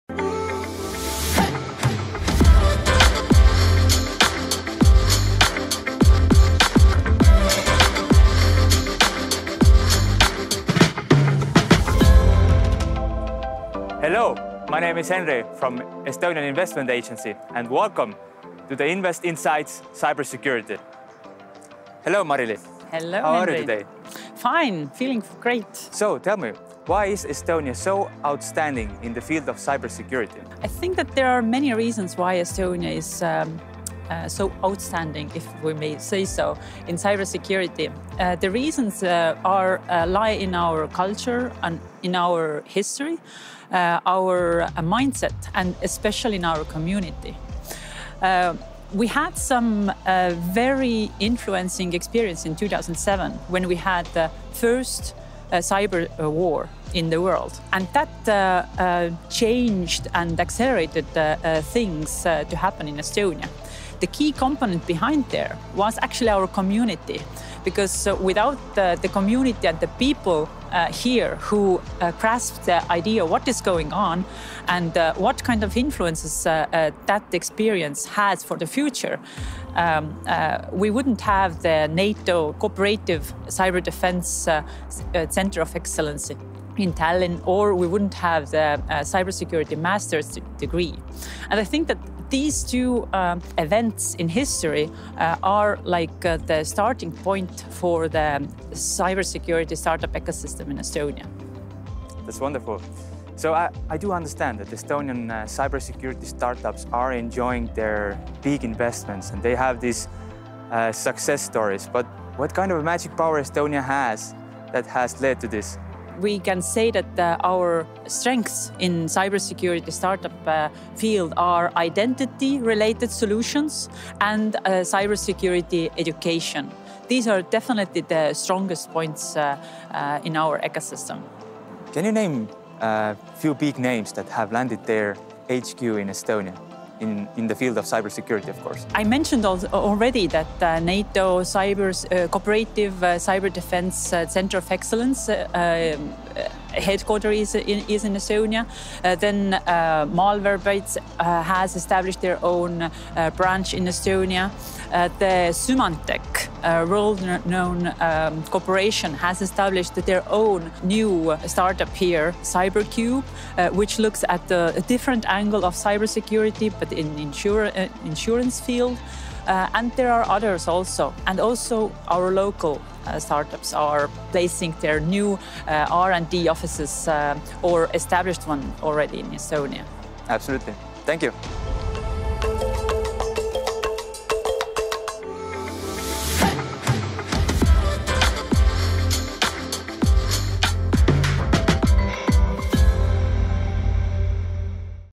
invEST insights: cyber security with Startup Estonia
Brief insight to Estonian cyber security ecosystem with Marily Hendrikson, the Cyber Security Project Lead in Startup Estonia and Henri Räim, the Deputy Head of Global Business Development at the Estonian Investment Agency.